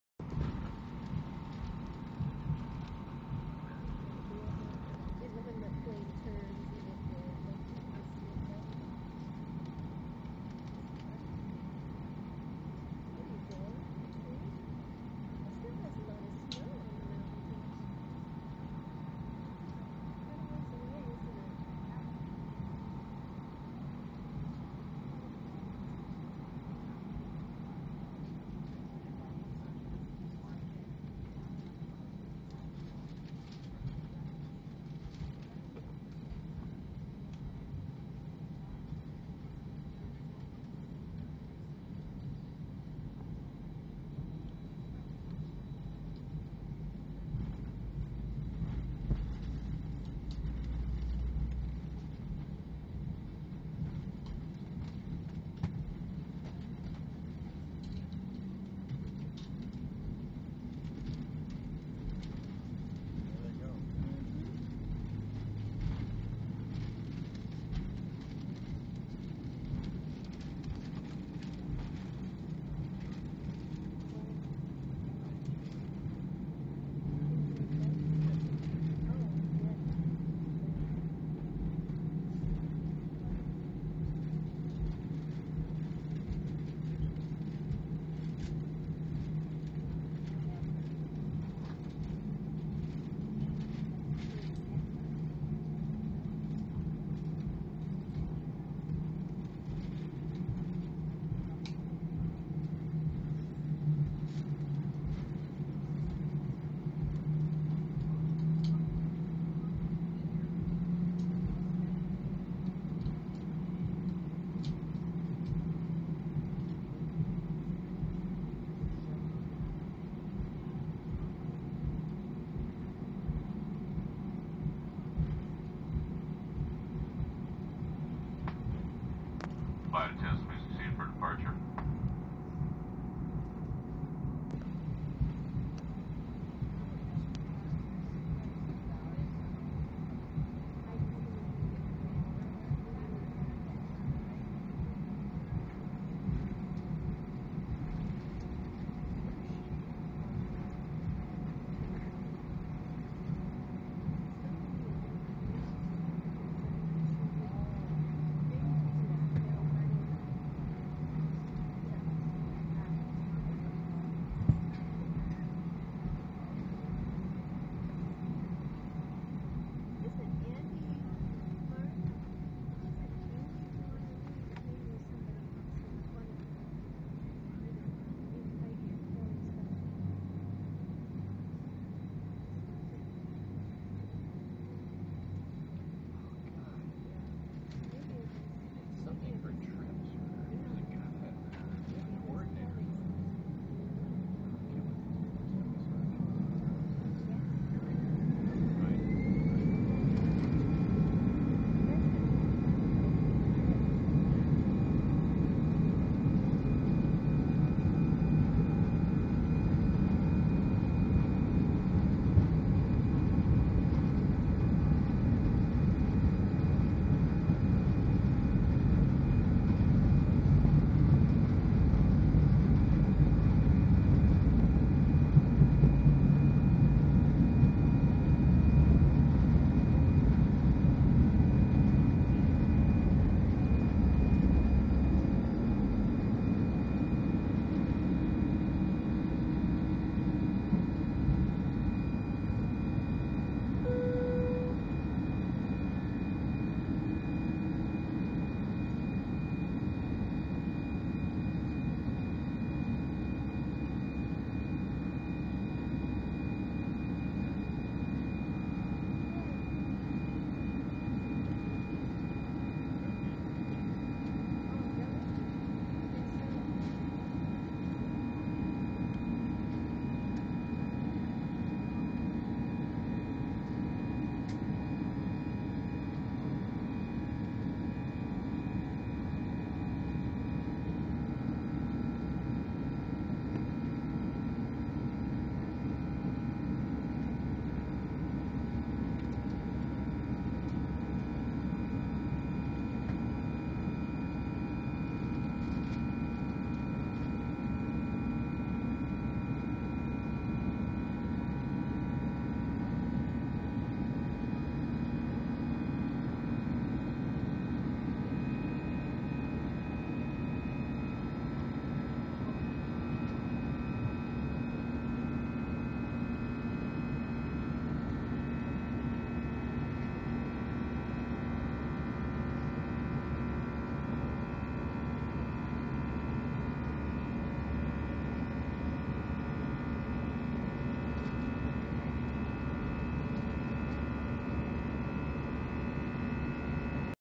Frontier A319 takeoff from Denver International Airport.
Frontier, A319, Denver... Hell yeah!  PS:  Sorry for low quality, my camera was out of memory, so the only thing I had left was my cell phone :/